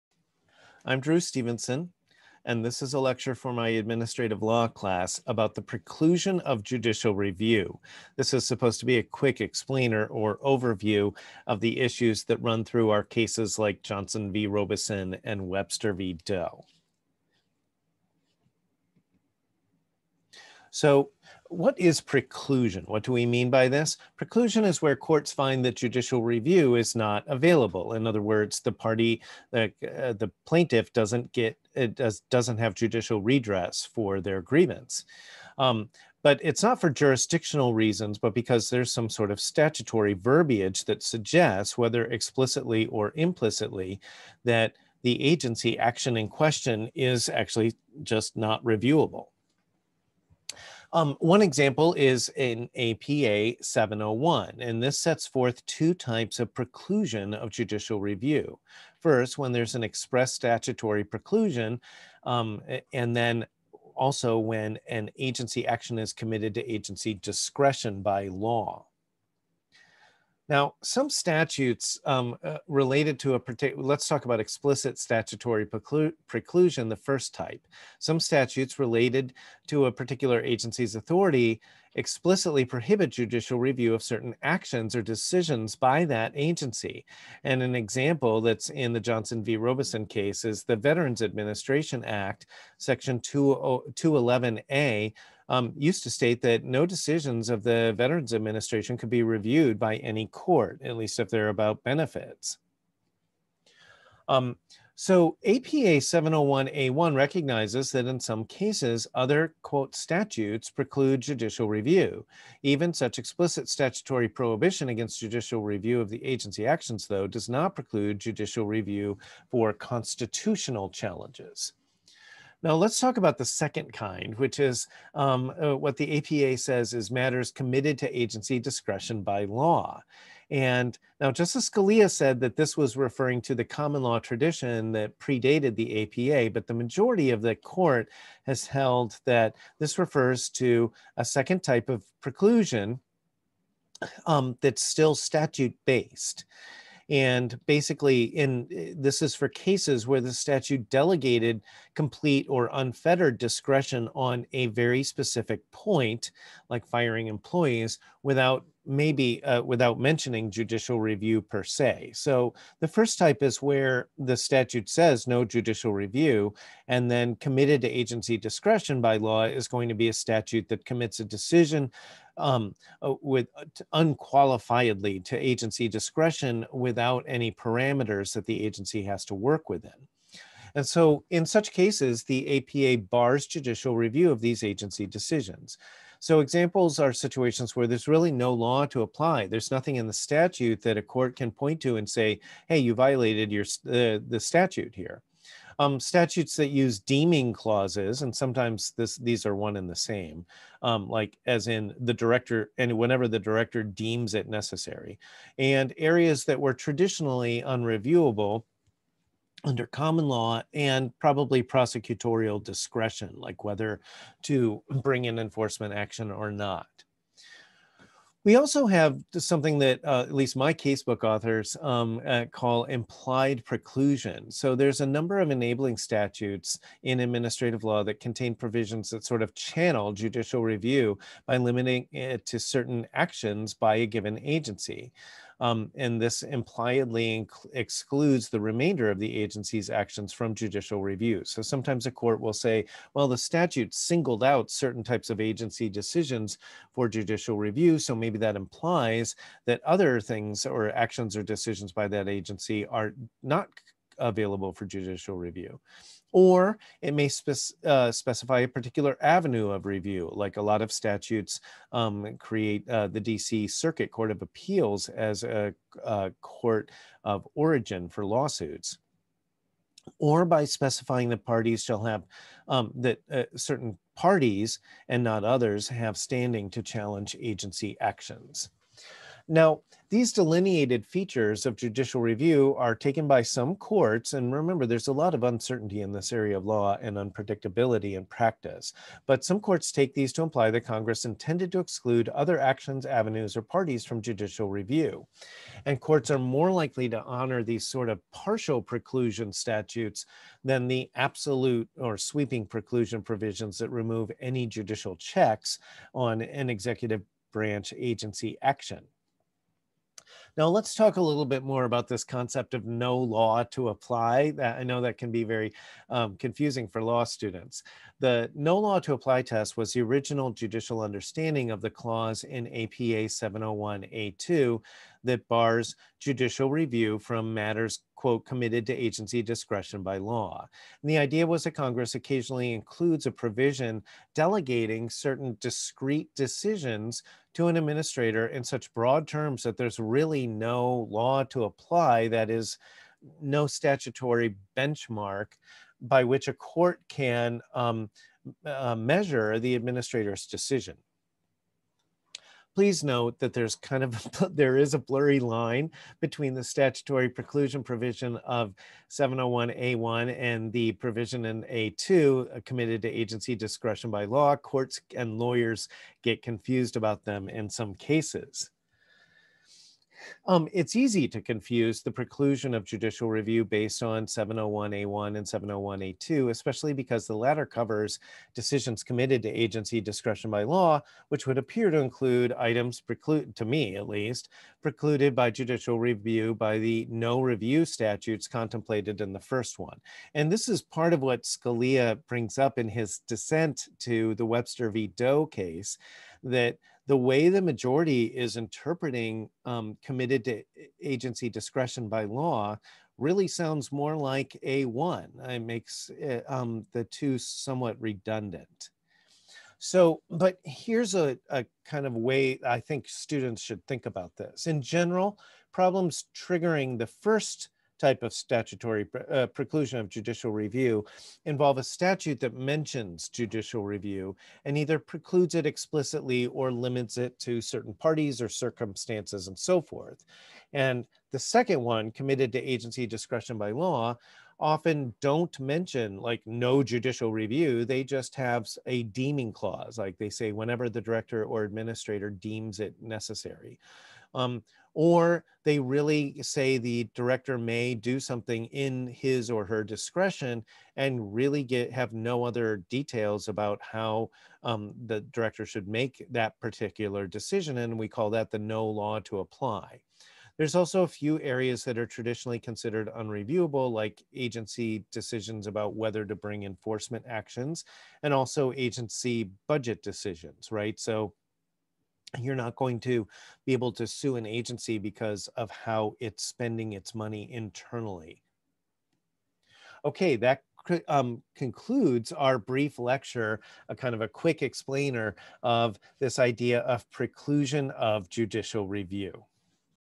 Preclusion of Judicial Review - Quick Explainer
Quick explainer video for my Administrative Law course about preclusion of judicial review under the Administrative Procedure Act, especially under Section 701(a).